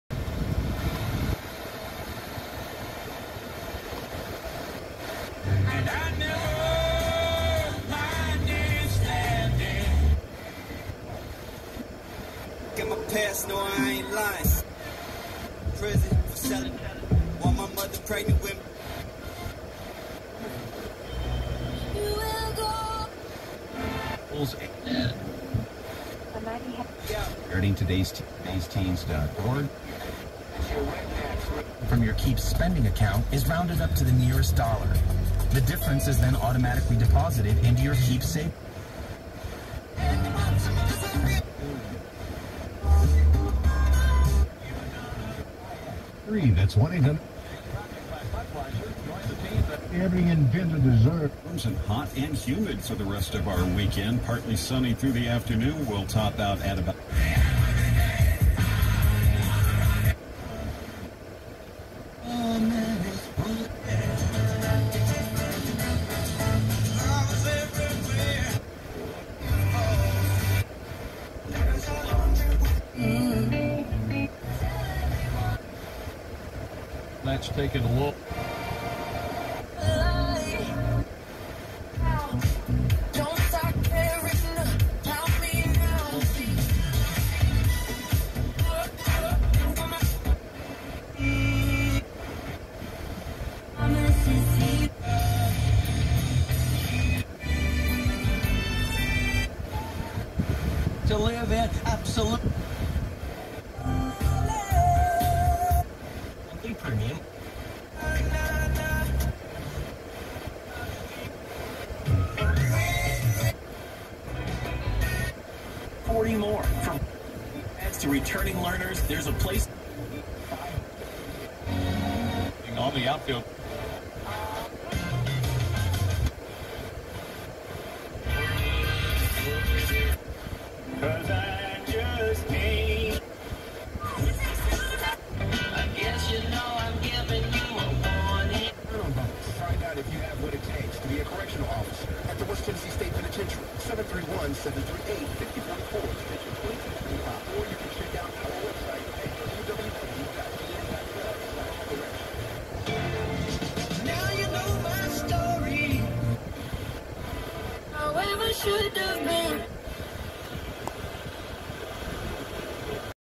Another FM bandscan from Fredericktown, MO (minus DX stations)
A scan of the FM band in Fredericktown, MO.